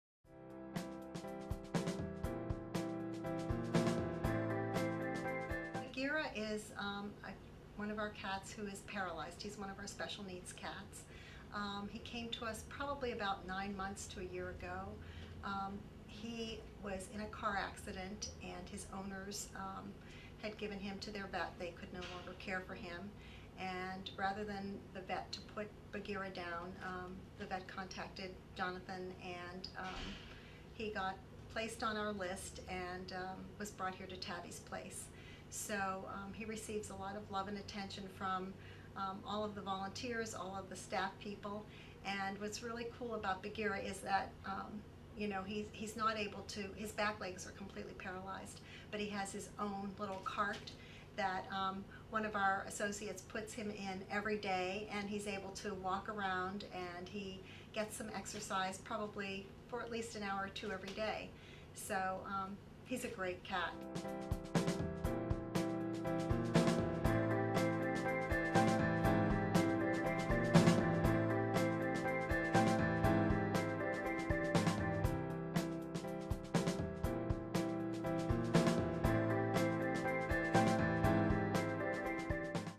Bagheera
Bagheera, a paralyzed cat gets a second chance!